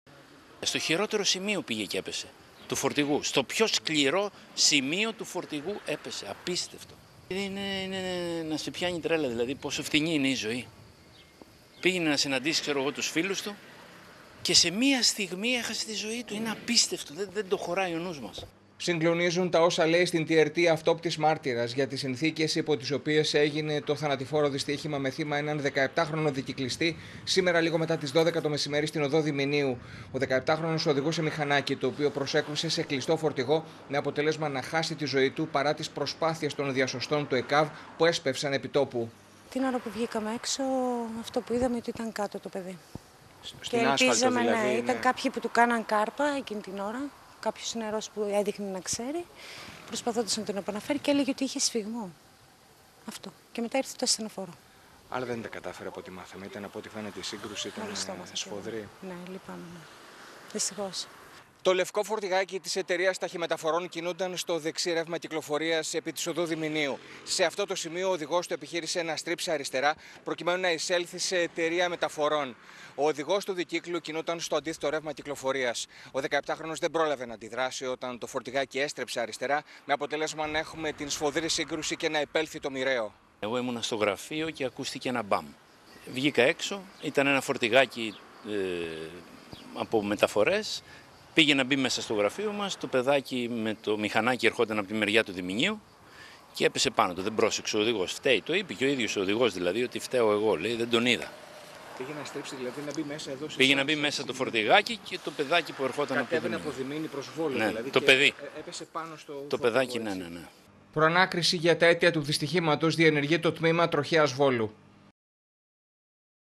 Βόλος Τραγωδία στην άσφαλτο – 17χρονος σκοτώθηκε στην οδό Διμηνίου 100723
Τραγωδία σήμερα στις 12 το μεσημέρι στην οδό Διμηνίου, στον Βόλο. 17χρονος οδηγός δικύκλου έχασε τη ζωή του, όταν το μηχανάκι του προσέκρουσε σε κλειστό φορτηγάκι, ο οδηγός του οποίου επιχειρούσε να στρίψει αριστερά για να μπει σε μεταφορική εταιρία. Παρά τις προσπάθειες που έγιναν επί τόπου για να κρατηθεί στη ζωή, ο 17χρονος δεν τα κατάφερε και άφησε την τελευταία του πνοή στο ασθενοφόρο του ΕΚΑΒ, το οποίο τον μετέφερε στο Νοσοκομείο. Σε κατάσταση σοκ ο οδηγός του κλειστού φορτηγού είπε ότι δεν αντελήφθη τον οδηγό του δικύκλου, ενώ συγκλονισμένοι για όσα είδαν να εκτυλίσσονται μπροστά τους, μίλησαν στην TRT αυτόπτες μάρτυρες του τροχαίου δυστυχήματος.